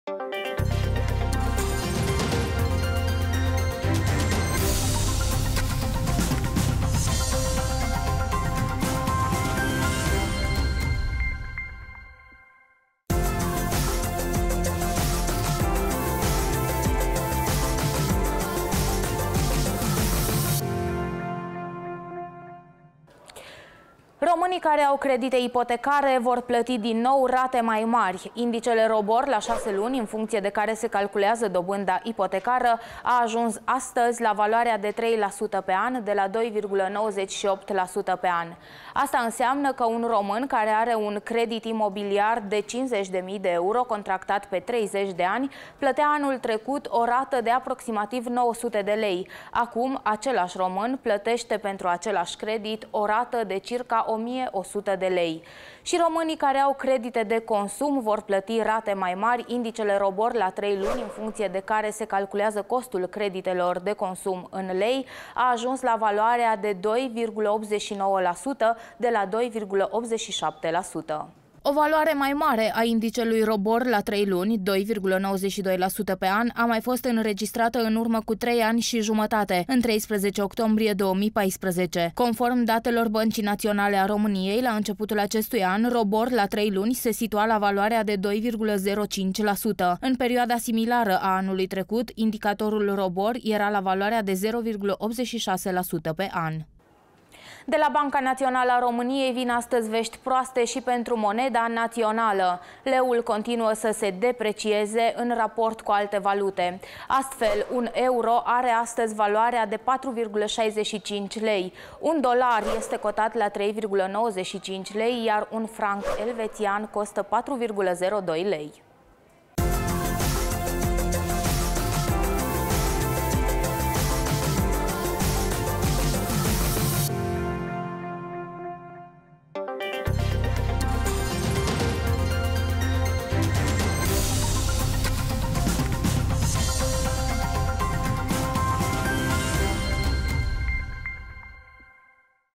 Money News - Leul, tot mai slab in fata valutelor
ROBOR își continua cresterea, iar leul este tot mai slab in fata valutelor. Aflati mai multe detalii din materialul urmator